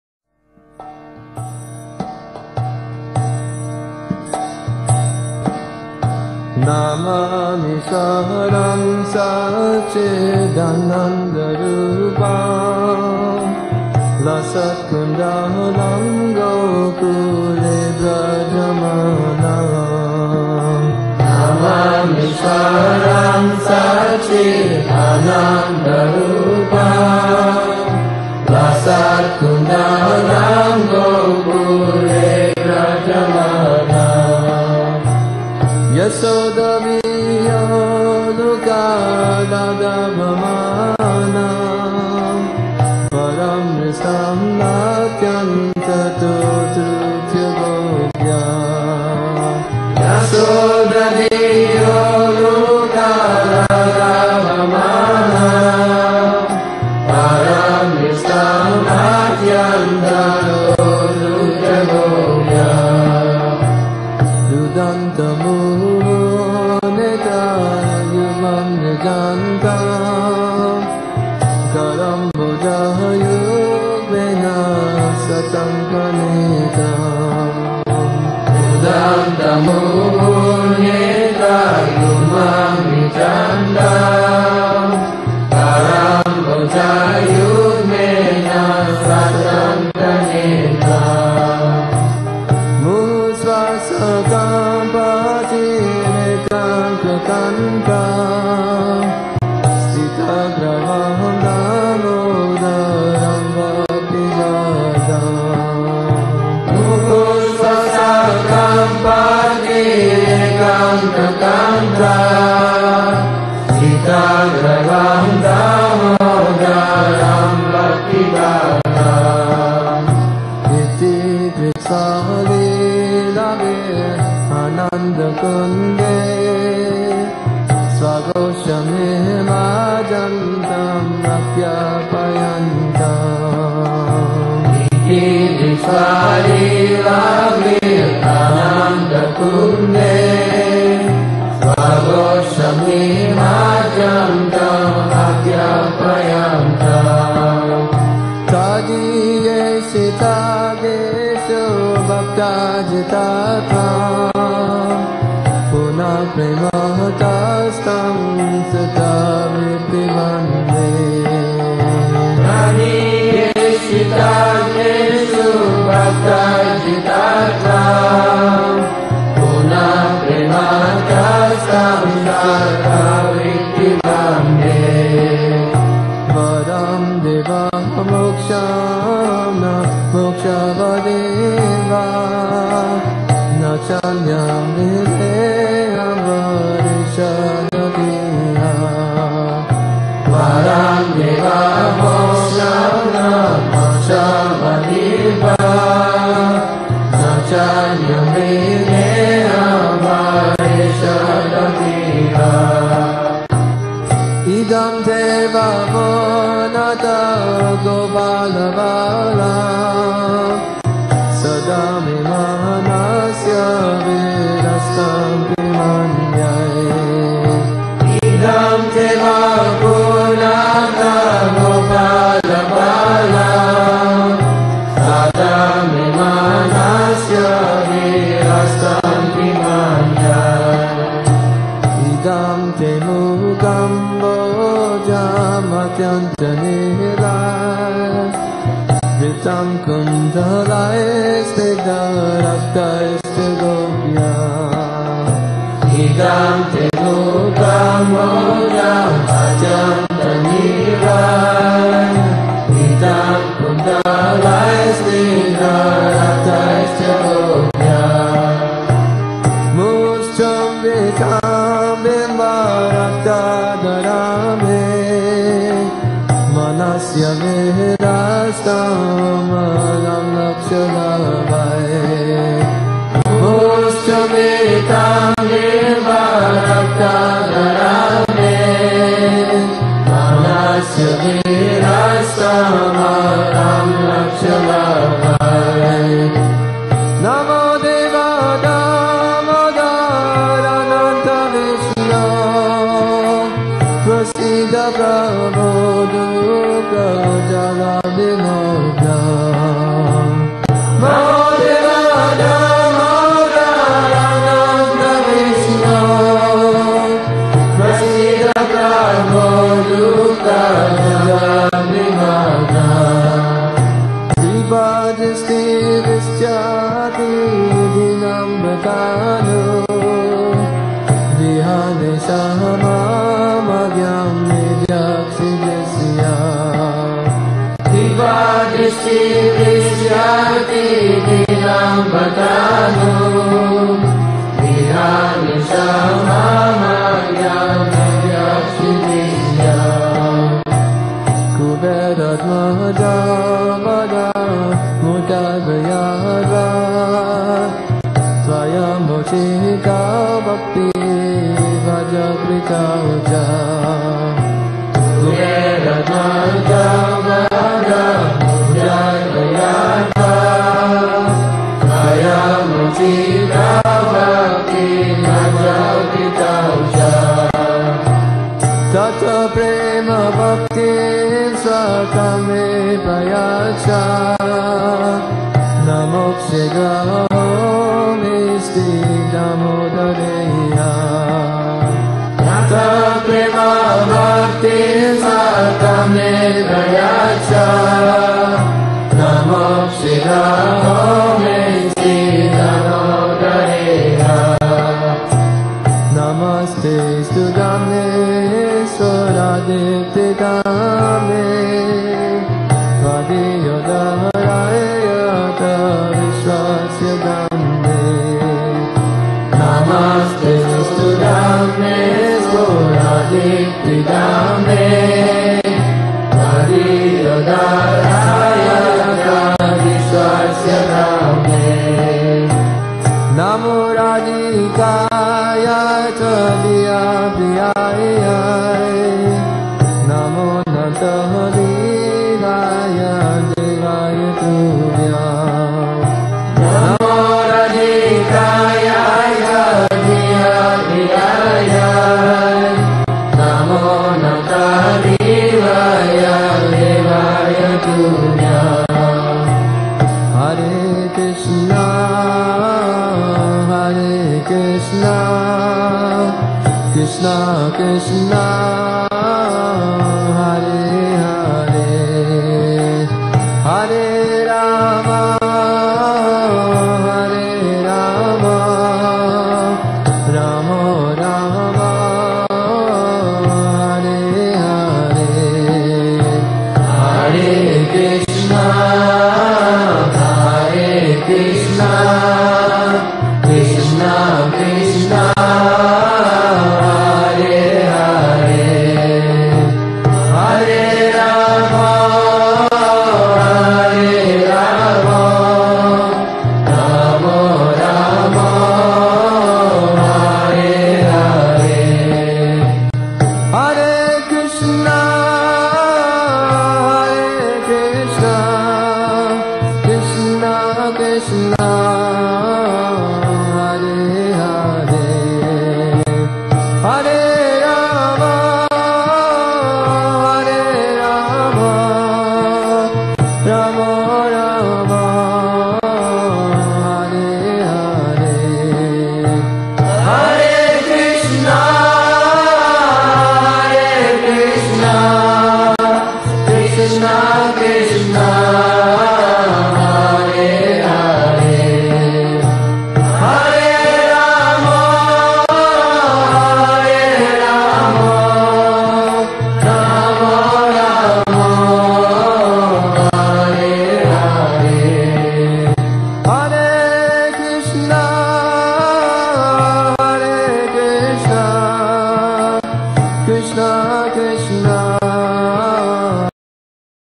Viaggio alle pendici dell'Himalaya (Centro Studi Bhaktivedanta)
Un viaggio per immagini alle pendici dell'Himalaya: il racconto tramite foto e musica di un Seminario organizzato dal Centro Studi Bhaktivedanta e tenuto da Marco Ferrini (Matsyavatar das) nelle città sacre di Rishikesh e Devaprayag dal 25 agosto al 4 settembre 2010.